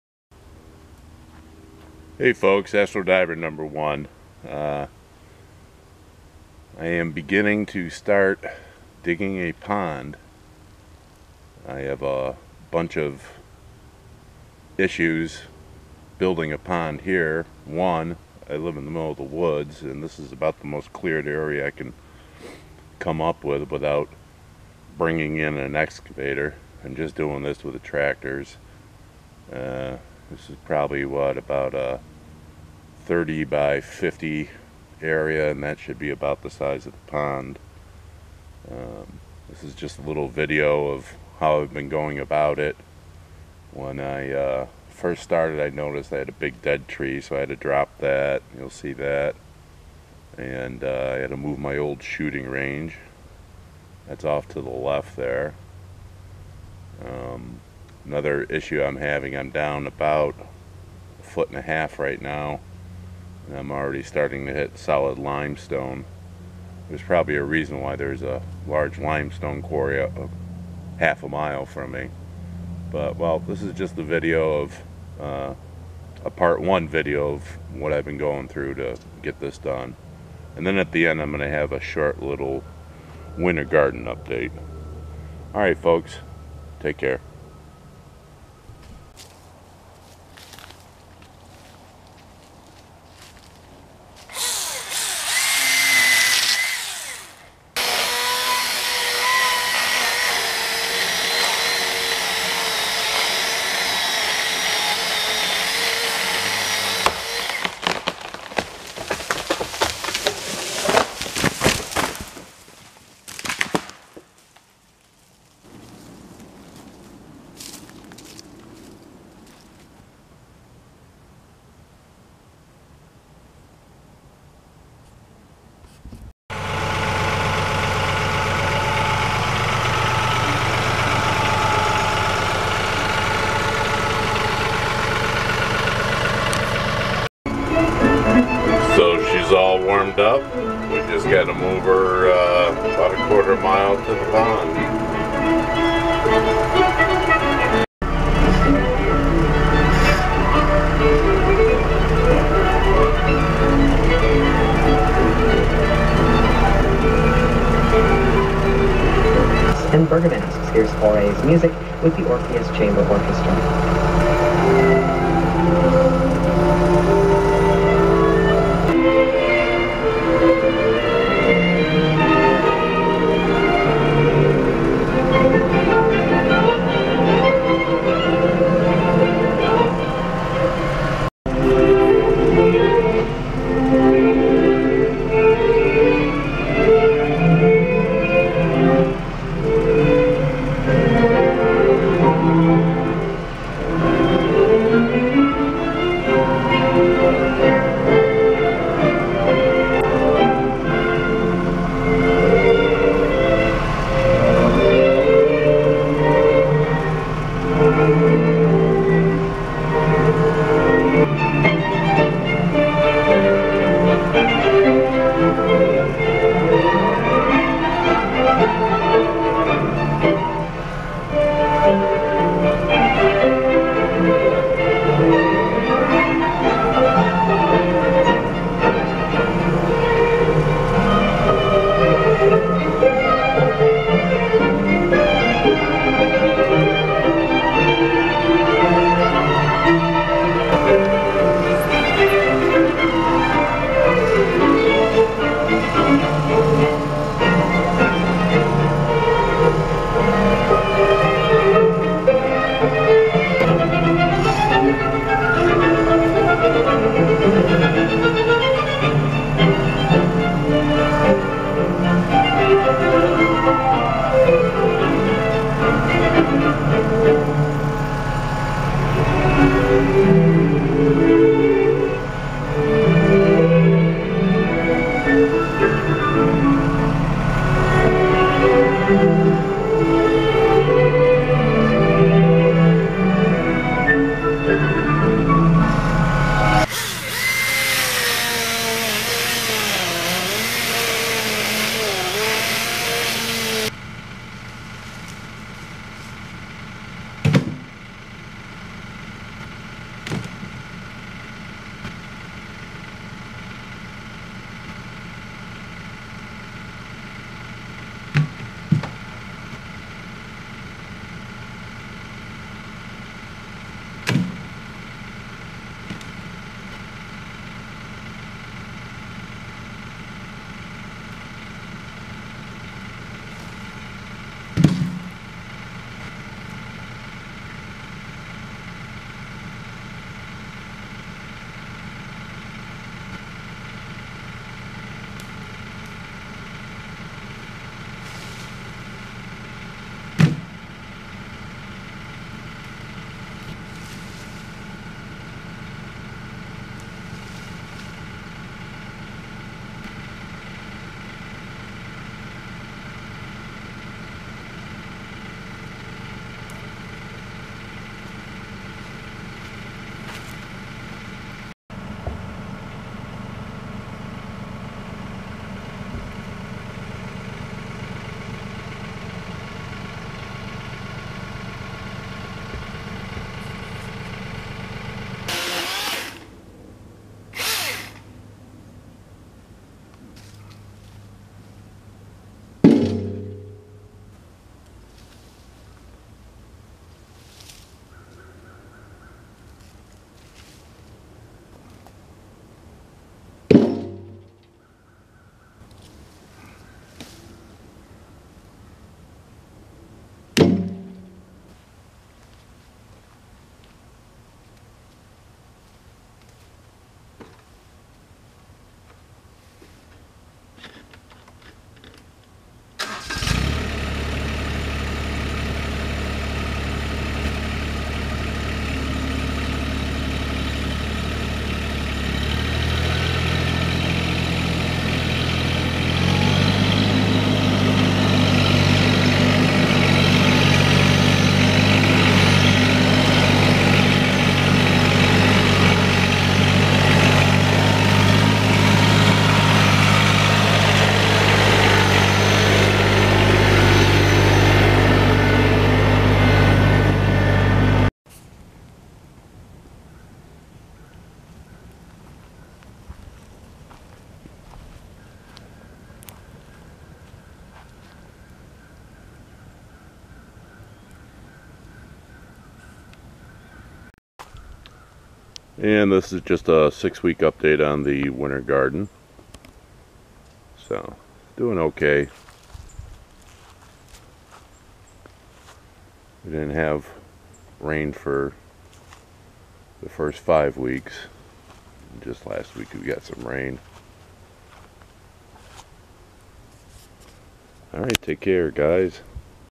Digging my first pond in the woods - part 1 and a 6 week winter garden update.
My 1st attempt at digging a 30 x 50 ft. pond in the woods.